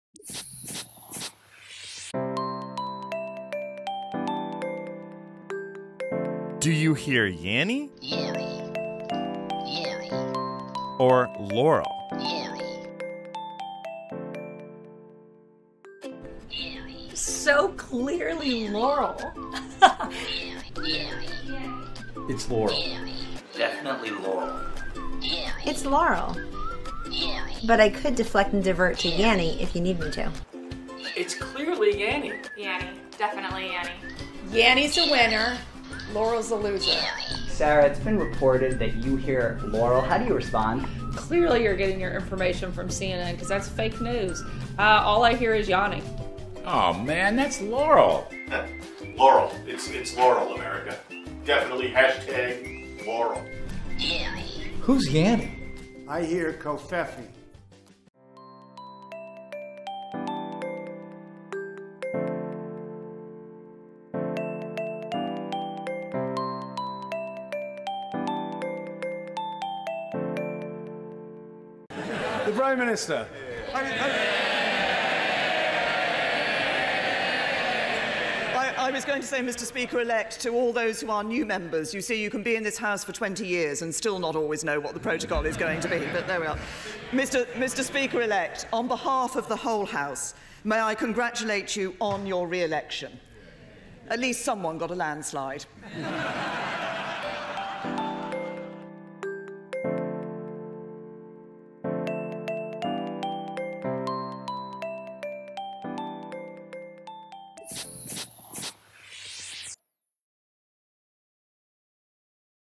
De kracht van zelfspot | Overtuigtip
Heb jij al meegedaan aan de internethype Yanny vs. Laurel? Ook het Witte Huis laat zich tot deze hype verleiden met als resultaat een compilatievideo waar ook Trump een rol in speelt. Een zeldzaam moment, want op de video toont hij zelfspot door te refereren naar zijn tweet van een jaar geleden: Convfefe. Een grappige video die laat zien dat zelfspot ook een handige tool voor een spreker kan zijn.